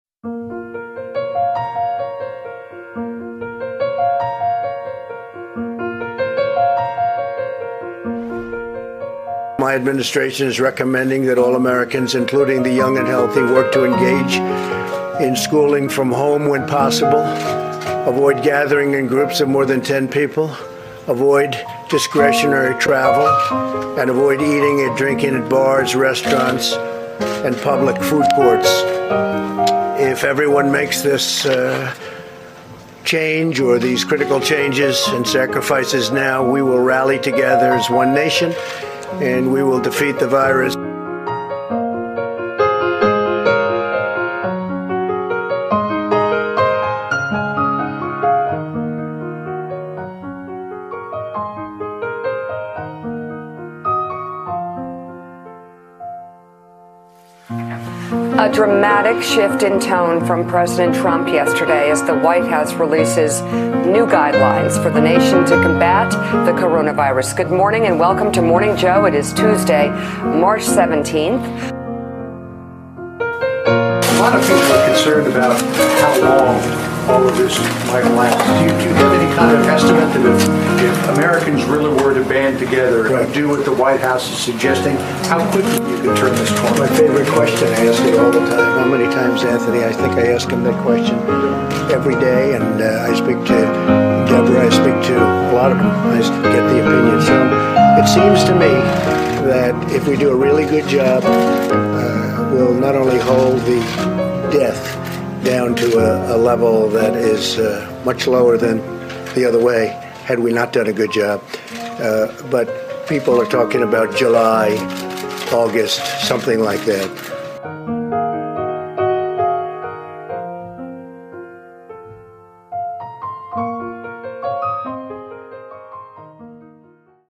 The CoronaVirus 61DayLockdown SC @shotbynae part 1 South Carolina
The CoronaVirus Impacts South Carolina

this video is used for public awareness, stock footage from story blocks have been provided, footage shot & edited by @ShotByNae